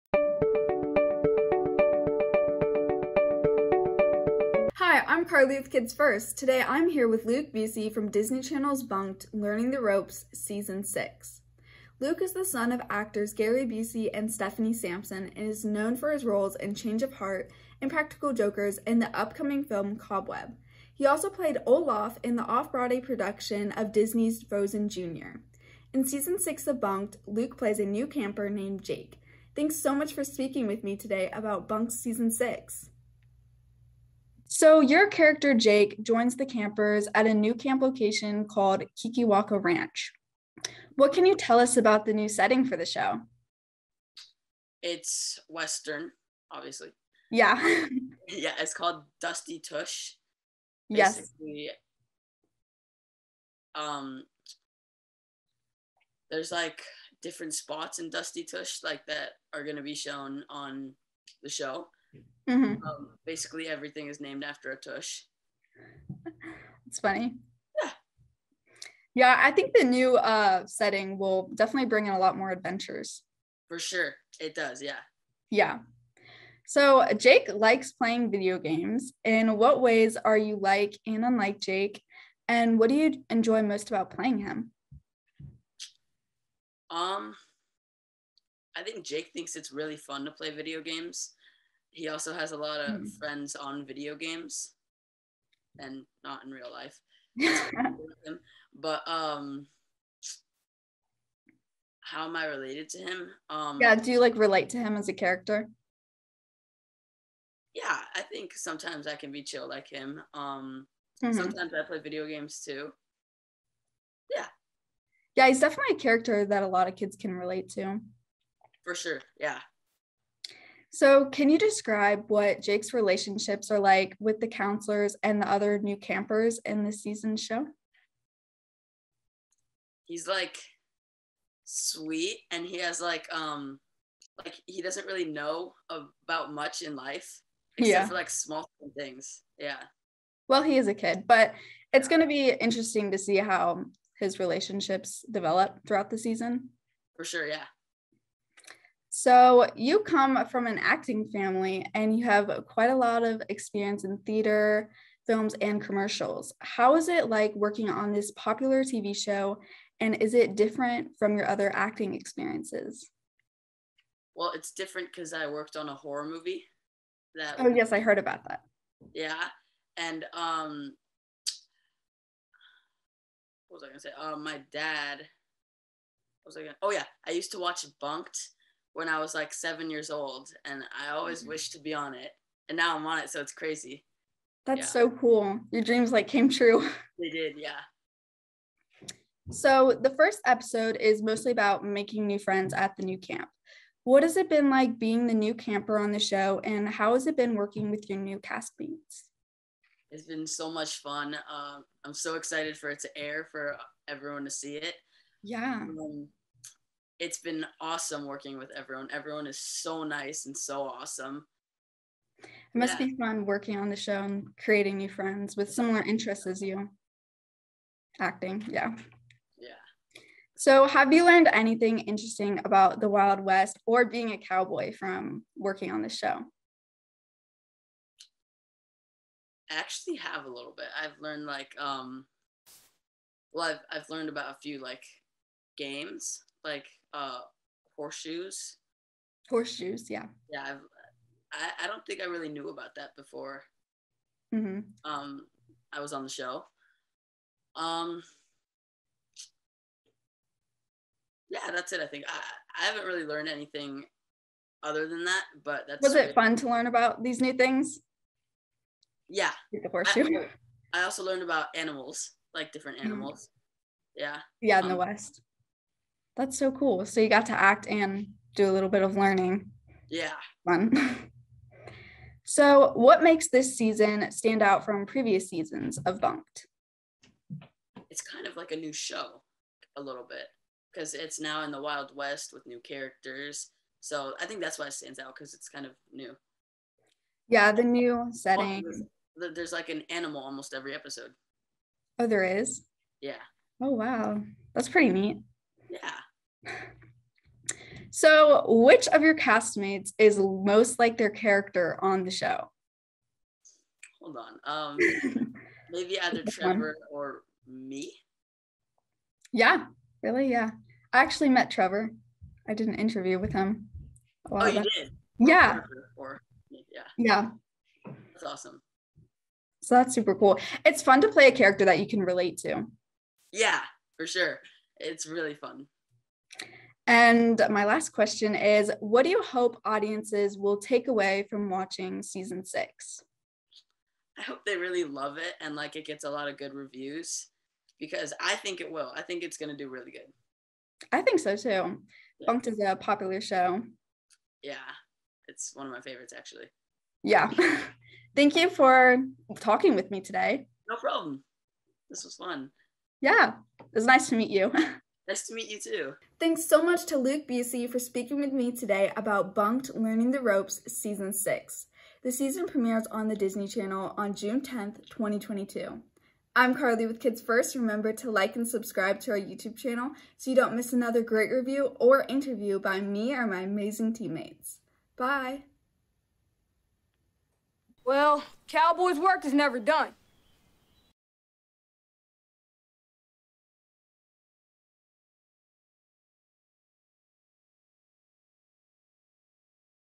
Enjoy Carlee S.'s interview with Luke Busey about Bunk’d: Learning the Ropes, Season 6
Enjoy Carlee S.'s interview with Luke Busey about Bunk’d: Learning the Ropes, Season 6. Luke is the son of Actors Gary Busey and Steffanie Sampson and is known for his roles in Change of Heart, Impractical Jokers, and the upcoming film, Cobweb. He also played Olaf in the off Broadway production of Disney’s Frozen Jr. In Season 6 of Bunk’d, Luke plays a new camper named Jake.

Bunk’d is back for Season Six with many exciting surprises. Bunk’d: Learning the Ropes welcomes familiar faces along with new campers to Kikiwaka Ranch in Dusty Tush, Wyoming for a Wild West experience.

The first episode starts off in a new setting called Kikiwaka Ranch where we learn that Lou (Miranda May, who is also director and executive producer) has opened a new Wild West camp to campers. However, Lou later reveals she isn’t exactly the legal owner of the camp, yet. This is a problem that she asks Parker (Trevor Tordjman) to help her with since new campers are arriving. Meanwhile, Destiny (Mallory James Mahoney) and Noah (Israel Johnson) are the camp counselors at the new camp. They are eager to meet their new campers, but soon realize that three new campers -- Jake (Luke Busey), Bill (Alfred Lewis) and Winnie (Shiloh Verrico) -- are not who they were expecting.

My favorite scene is when the new campers Winnie, Jake and Bill set off on an adventure alone.  Bill is distracted and falls deep into an abandoned mineshaft. Without hesitation, Winnie and Jake jump in to try and save Bill. Alone in the cave they realize that no one is around to save them. The suspense of the scene and getting to see the new campers work together to try and escape before the cave collapses is entertaining. The Wild West setting is a new refreshing addition to the series. The décor of the camp and the addition of the mine help viewers get a sense of the Wild West without being overdone. The new campers are relatable to many viewers. Each one has a personality that adds interest to the show.  Their outfits fit perfectly with their personalities. Bill is a young cowboy trying to uphold a family legacy whose clothes fit perfectly the theme of the show. Luke is a laid-back gamer trying to find enjoyment at camp without video games. Winnie is daring, yet kind- hearted and wears edgy clothes. In contrast, her counselor, Destiny, is affectionate and very stylish. Her bright clothes and curled hair display that she is really into fashion.

The messages in the show this season are about building friendships and accepting others for who they are helps people grow. The campers each have their own style and their own strengths, which make them unique. Despite their differences, the bonds they create working together as a team help them form friendships.

I give Bunk’d: Learning The Ropes, Season Six, 5 out of 5 stars and recommend it for ages 8 to 14. Season six premieres June 10, 2022 on Disney Channel.